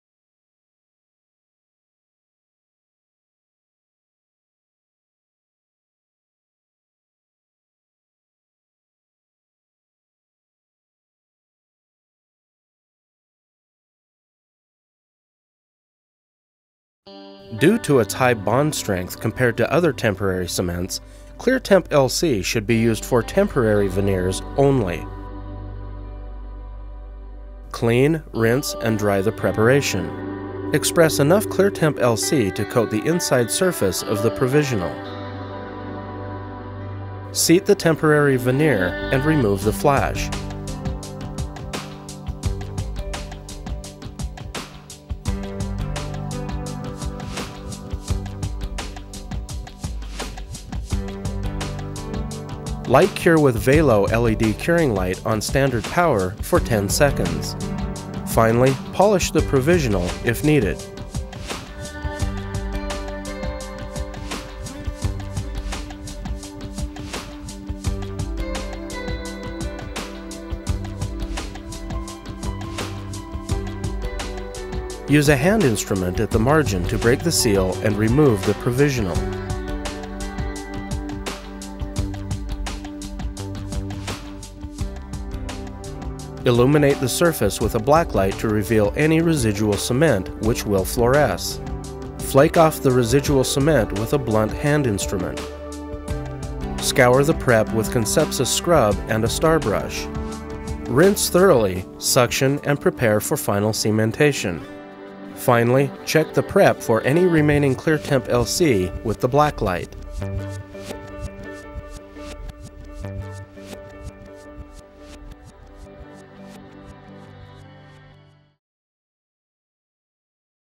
Temporary Veneer Placement & Removal with ClearTemp LC
ClearTemp LC is a luting material designed specifically for luting temporary veneers. Its proprietary, light-cured resin formula provides the additional strength required to keep provisional veneers in place. ClearTemp LC's translucent shade provides a natural look, while its fluorescent properties allow for easy detection and complete removal.